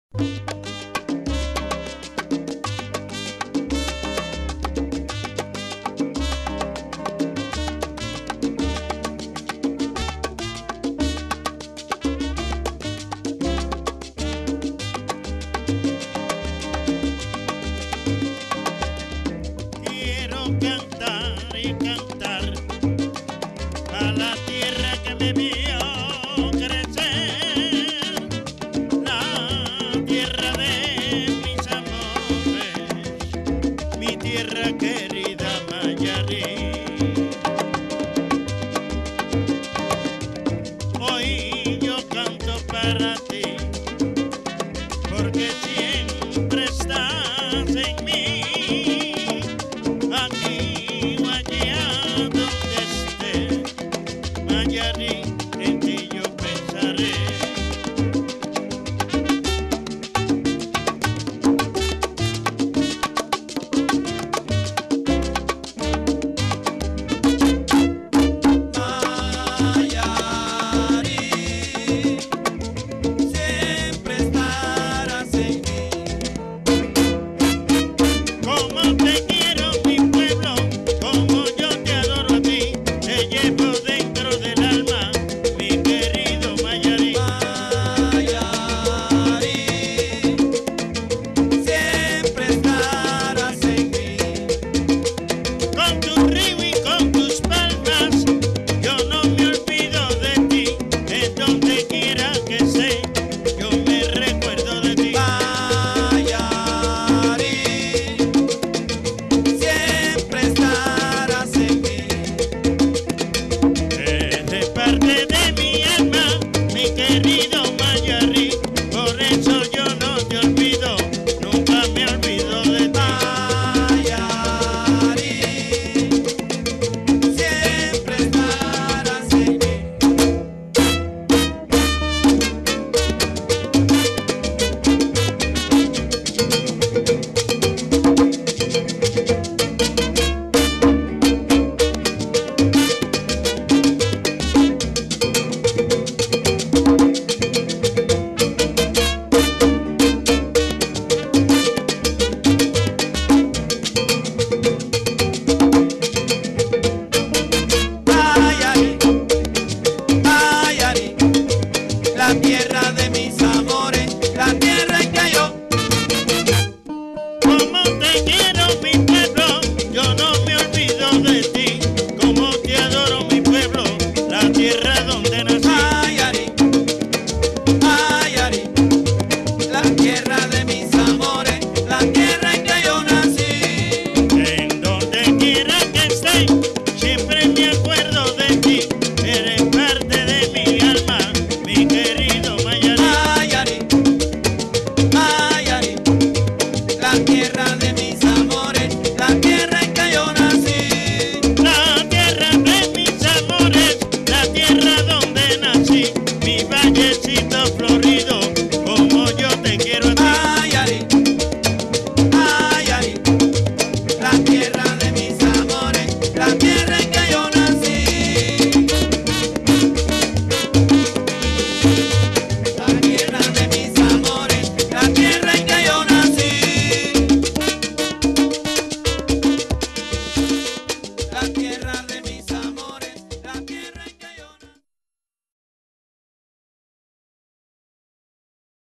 especial son
Especáculo inaugural del XXIII Encuentro de agrupaciones soneras de Mayarí. Jueves 7 de noviembre de 2013. Fragmento.